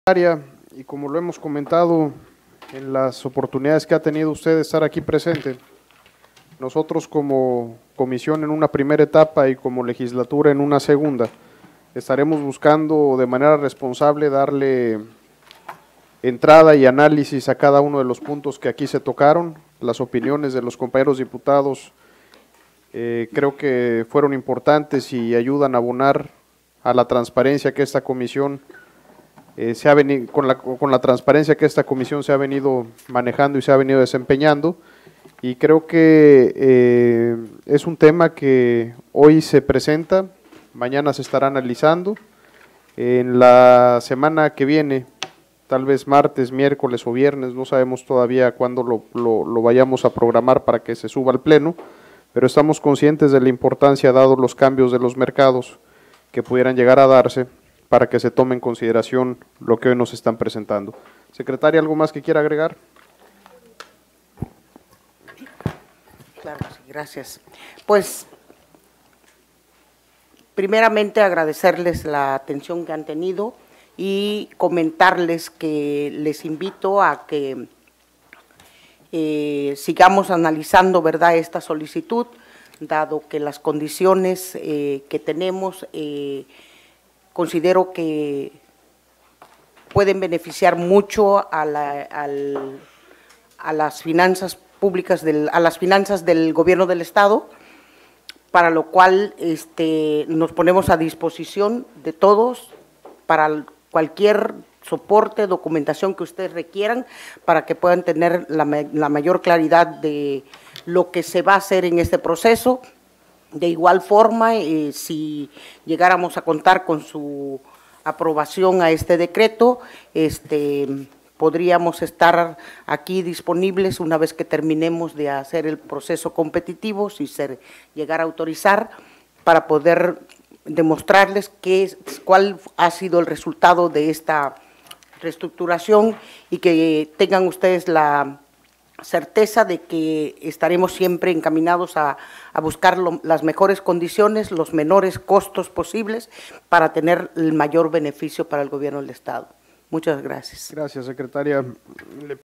Intervención del Dip. José de la Peña Ruiz de Chávez. PVEM (5/Dic/2019)
Intervención del Diputado José de la Peña Ruiz de Chávez, del Partido Verde Ecologista de México, durante la reunión de la Comisión de Hacienda, Presupuesto y Cuenta de la XVILegislatura para el análisis de la Iniciativa presentada por el Gobernador del Estado en materia de Refinanciamiento de la Deuda Pública Directa de Largo Plazo